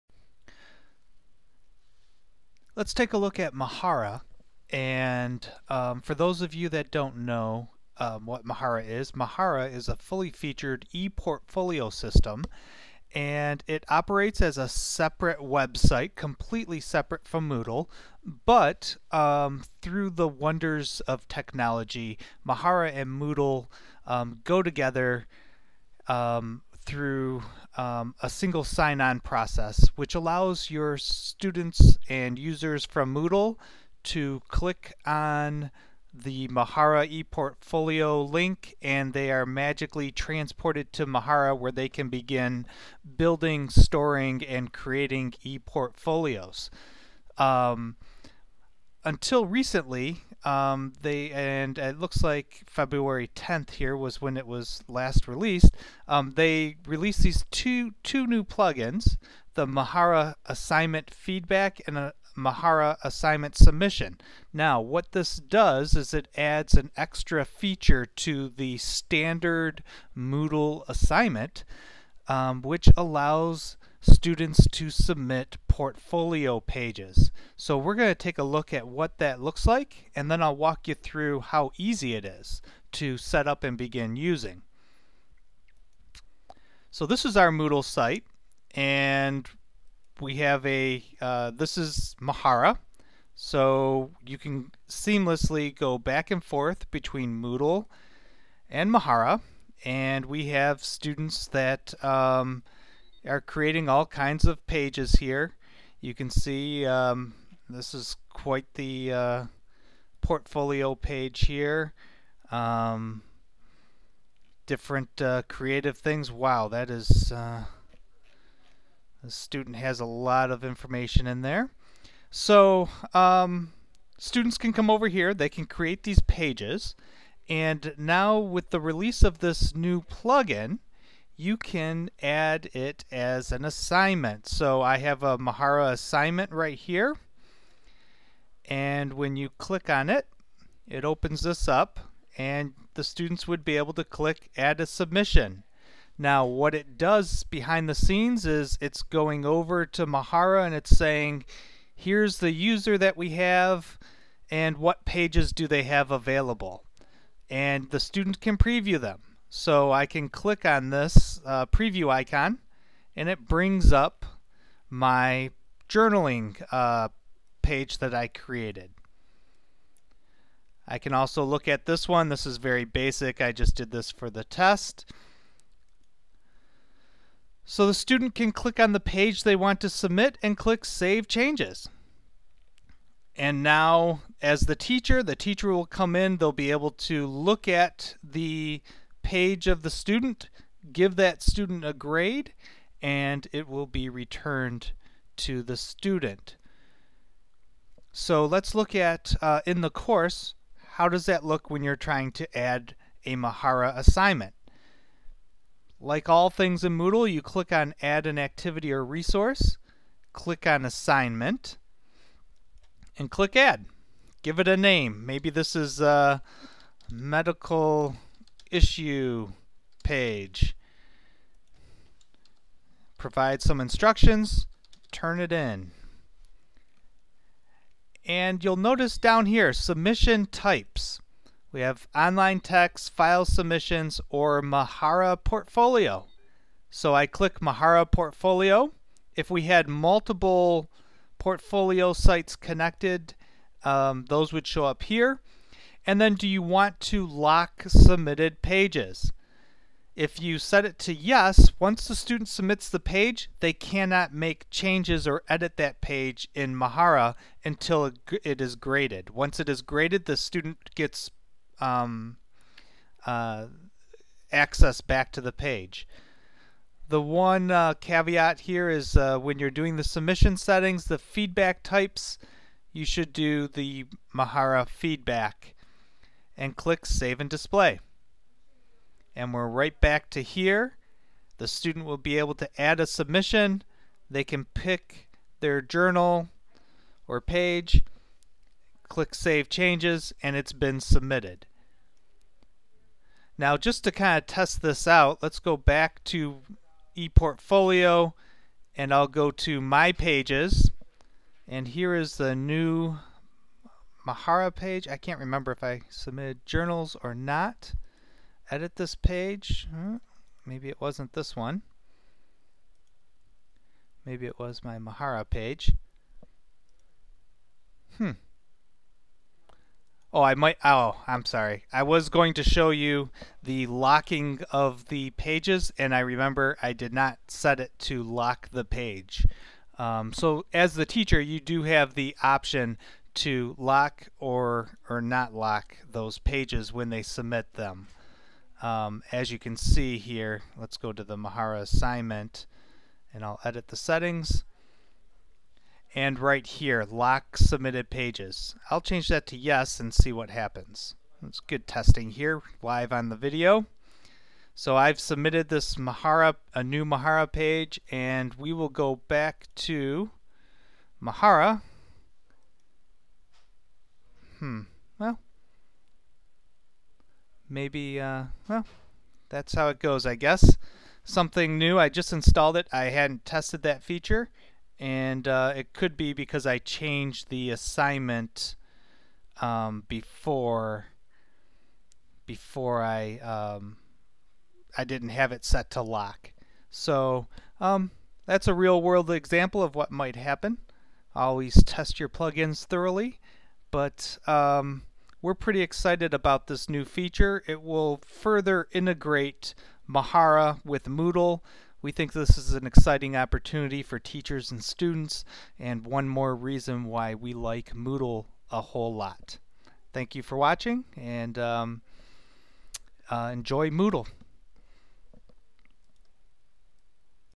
Mahara Moodle Assignment Submission
Allow teachers to grade Mahara Portfolio Pages within Moodle.
In the video there was confusion about the page locking feature.  I did further testing and can confirm it works as expected by locking the page until it is graded and then releasing the page back to the student.